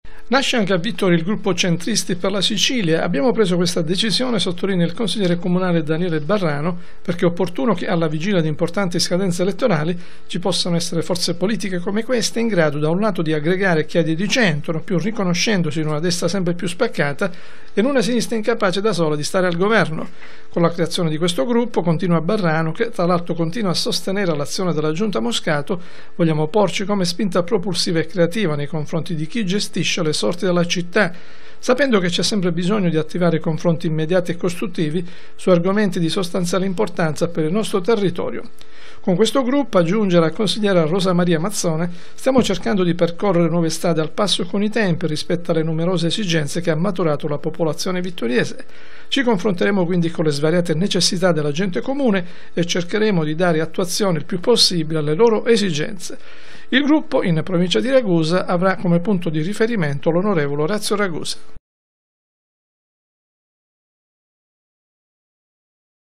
NASCE IL GRUPPO “CENTRISTI PER LA SICILIA”
- Anche Vittoria avra un gruppo di Centristi per la Sicilia. A livello regionale si rifa alle posizioni dell'on.Giampiero D'Alia mentre in ambito provinciale il riferimento e l'on.Orazio Ragusa.